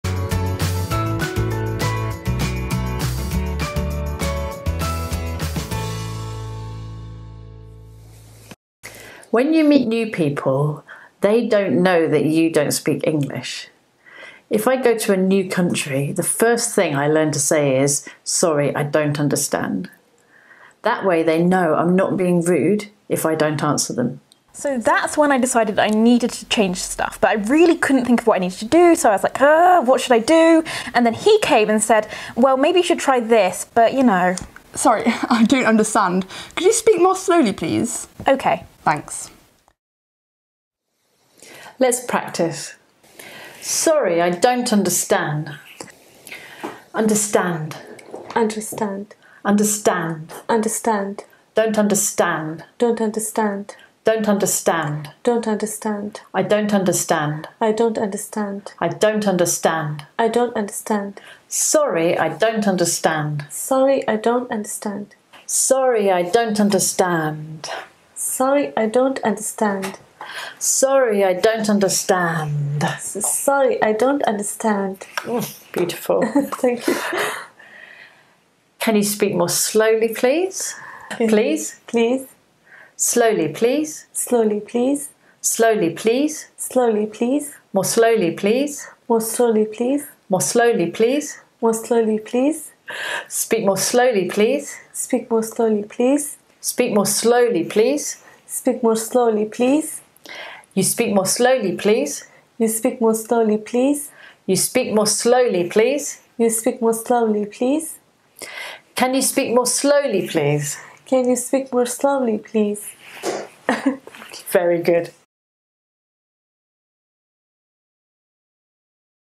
Îmi pare rău, nu înțeleg: Sorry I don't understand (Romanian)
Învață cum să le spui oamenilor când nu înțelegi ceea ce spun - o parte foarte importantă a limbii pentru noii vorbitori de engleză!
Learn how to tell people when you don't understand what they are saying - a really important bit of language for new speakers of English!